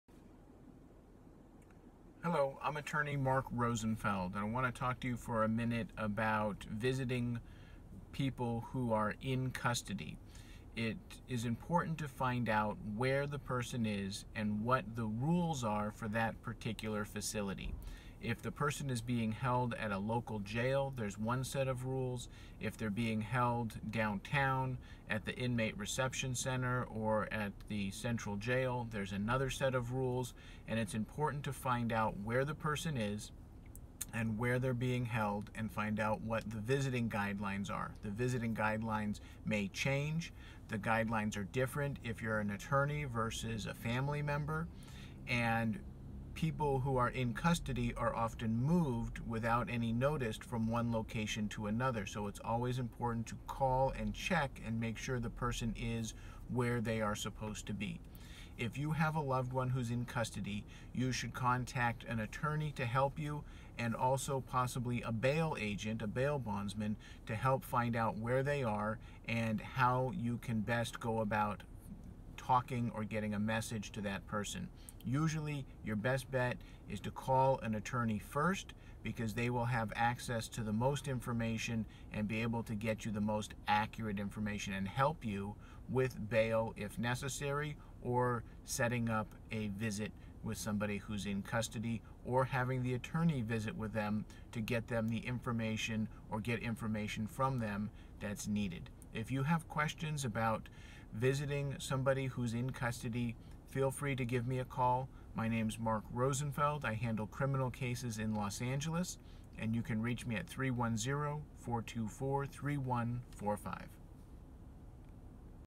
Jail Visits
Attorney Mark Rosenfeld talks about doing jail visits.

8200 Wilshire Blvd
Suite 200

Watch Los Angeles  Attorney, "MR DUI LA" Mark Rosenfeld as